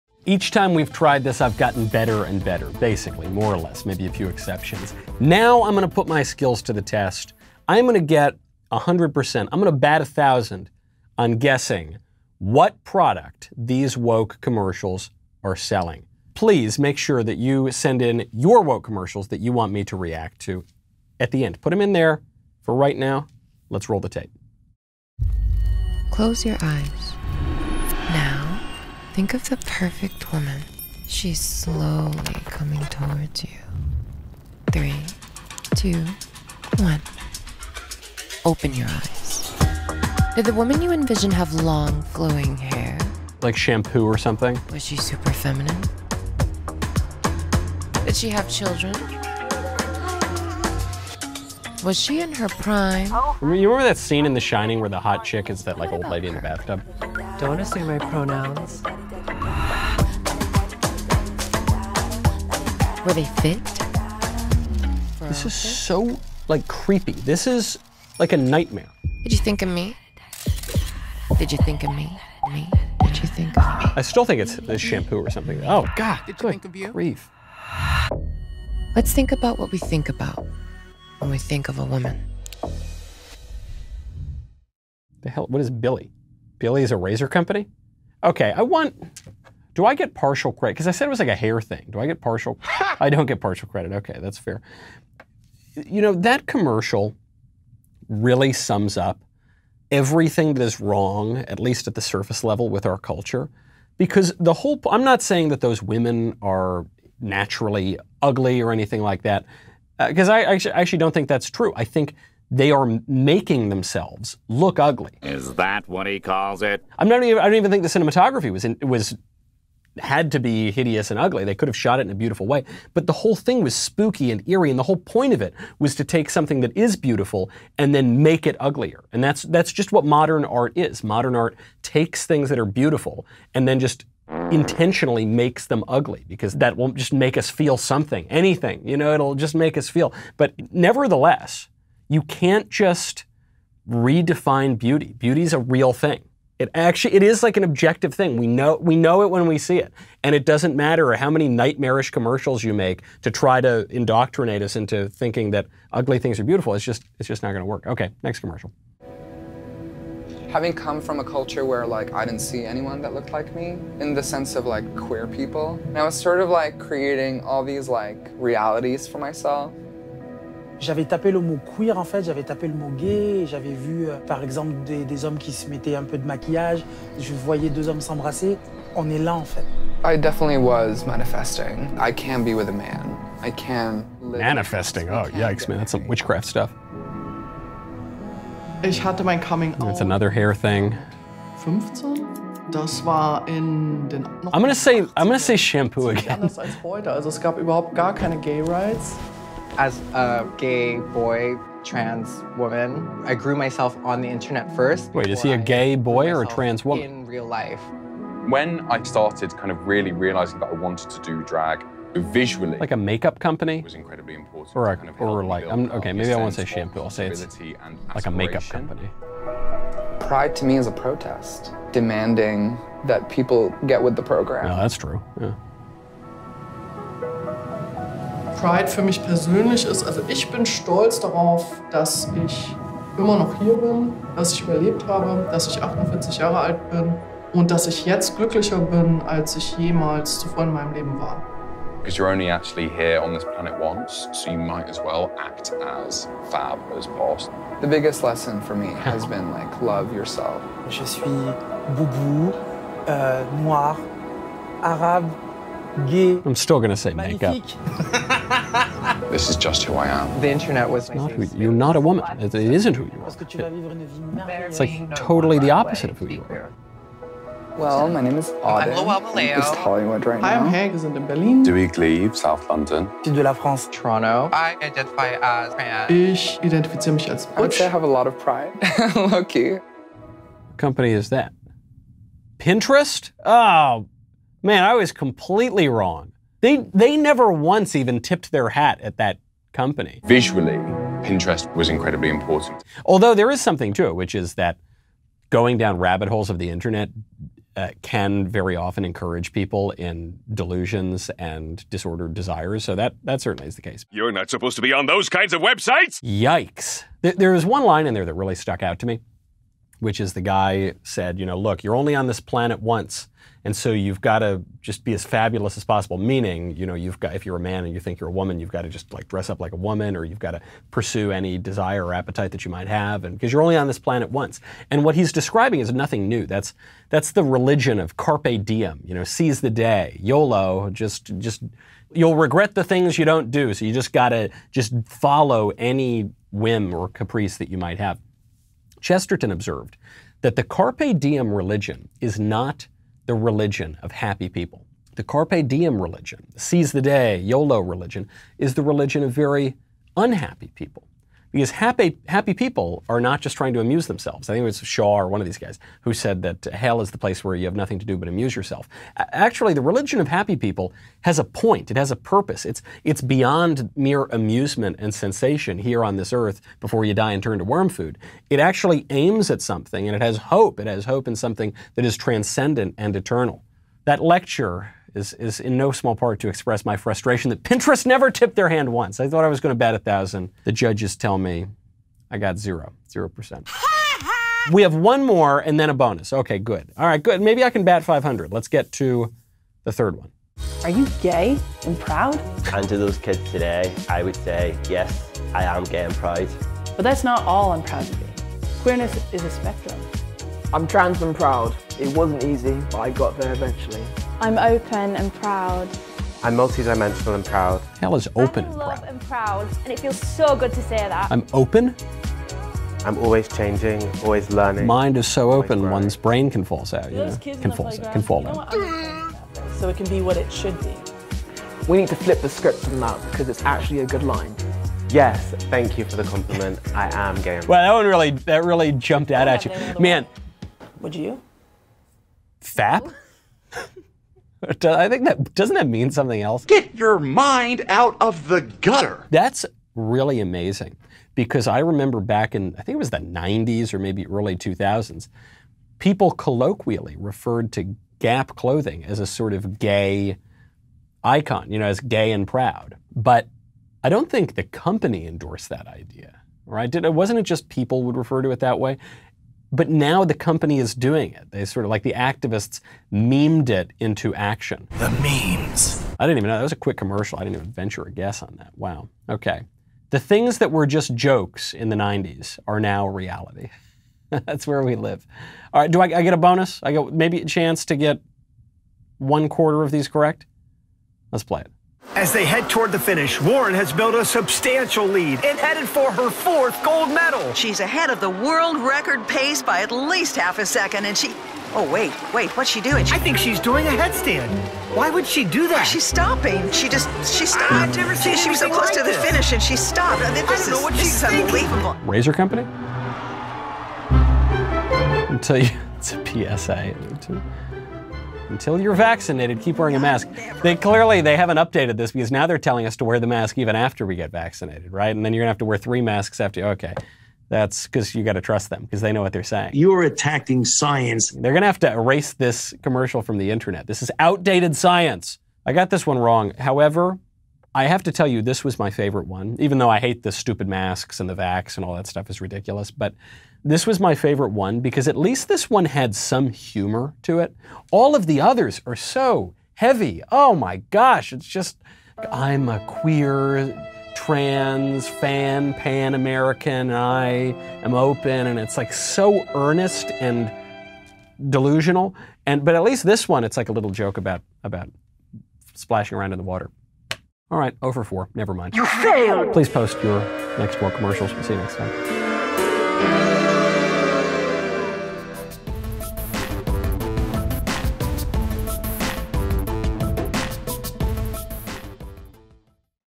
VIRTUE SIGNAL Commercials with NO Point | Try To Guess!
More viewers want to see if I can guess what these woke commercials are trying to sell and it's time to address the new commercials flowing in this year! These advertisements get more ridiculous by the day. Try to Guess!